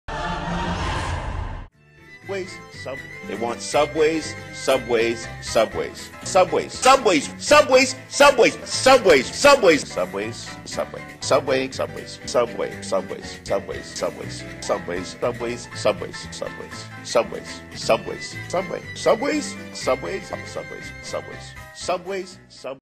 ROB FORD SHOW Ep 3 Subways Subways Subways
Episode 3. Rob Ford doesn't understand anything other than the word subways. He is such a smart man. Subways subways subways